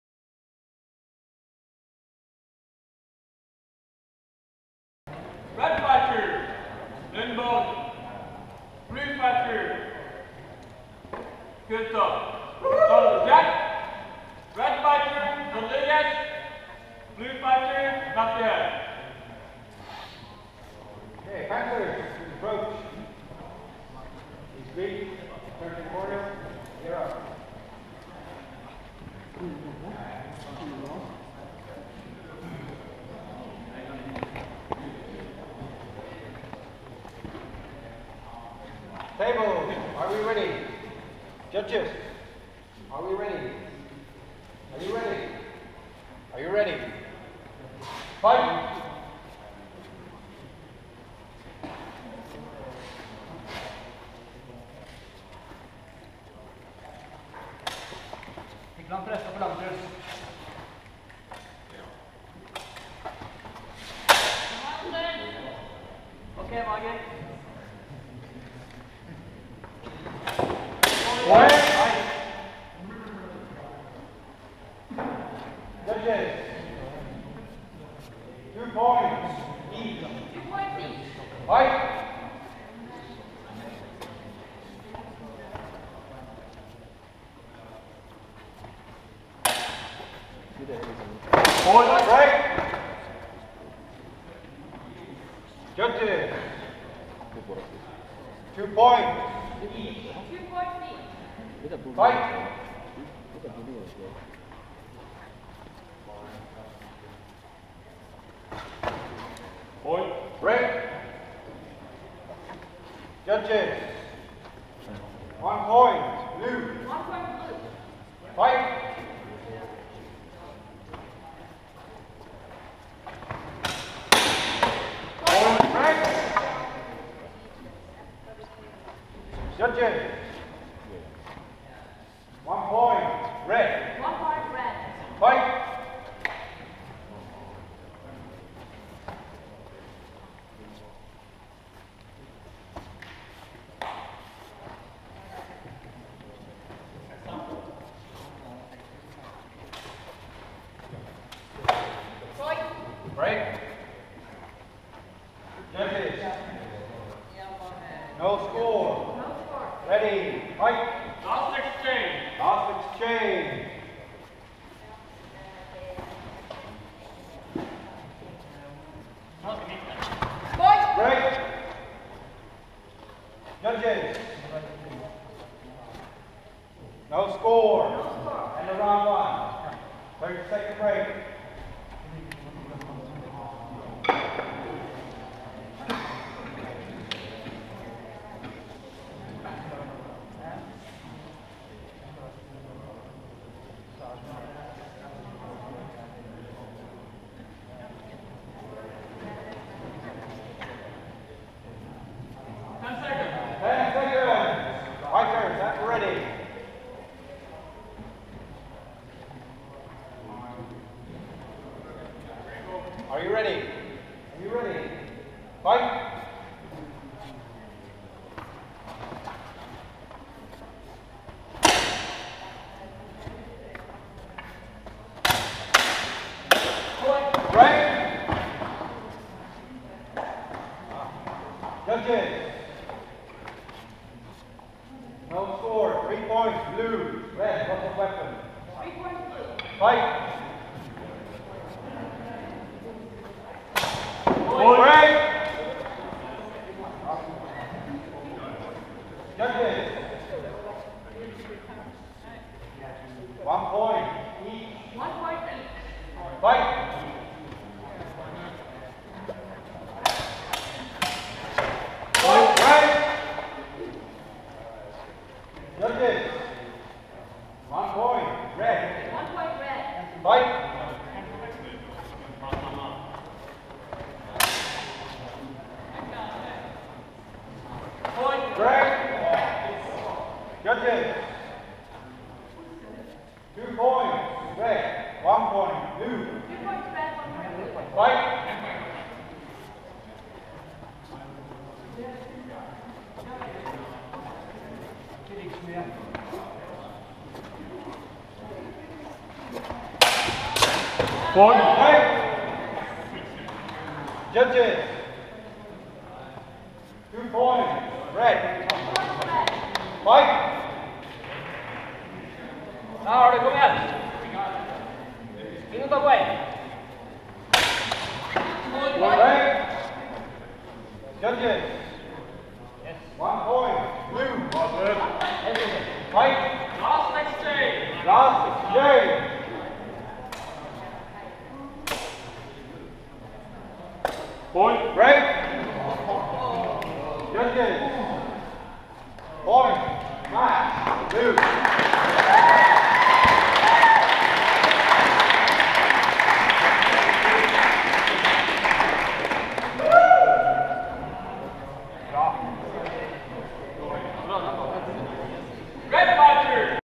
Bergen Open Longsword, Round of 16. M. Lundborg (R) vs. T. Kultorp(B)
A match from the Longsword tournament at the Norwegian HEMA event Bergen Open in 2014.